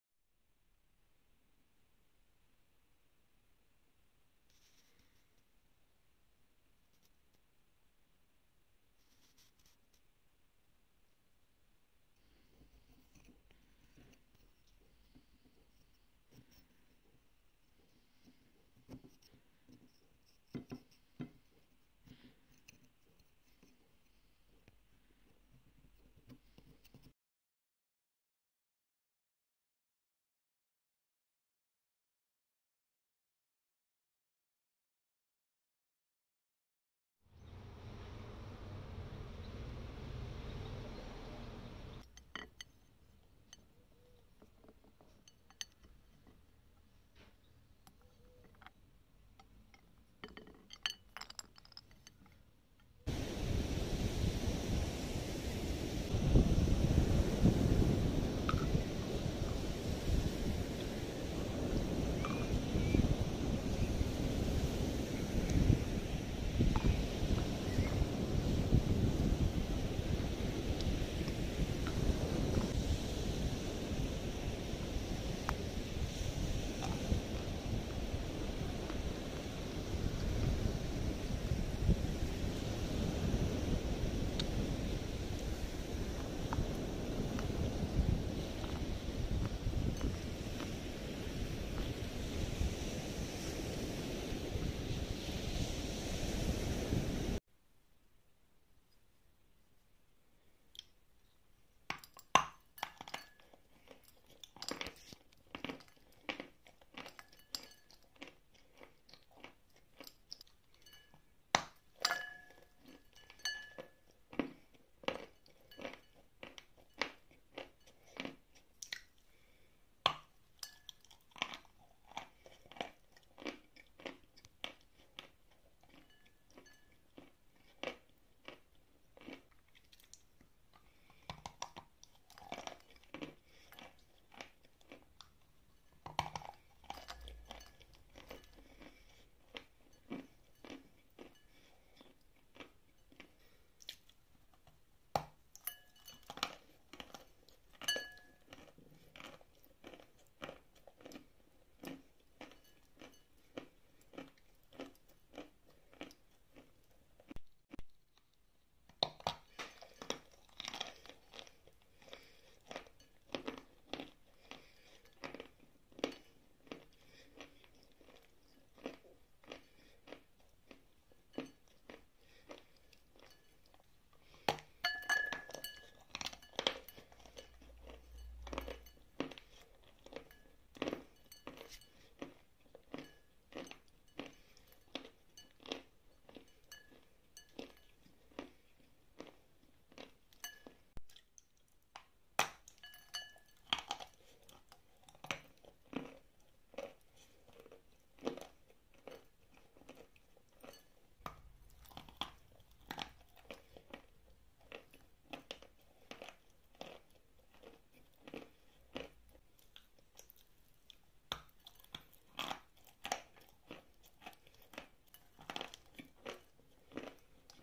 Виноград Зефир Безымянный. Мукбанг//Mukbang
Всем Привет.
Сегодня у нас Виноград.🍇
Вырезала его почти 6.5 часов.

Лорд Волан-де-Морт, то чье имя нельзя называть
Безымянный. Получен мной в розыгрыше.
Бубочки сделала из  Песчанки.
Покрасила акварелью Льва) (краски на основе мёда вкус не чувствуется)
Мягкий классический  Сухой. Побелочный  вкус.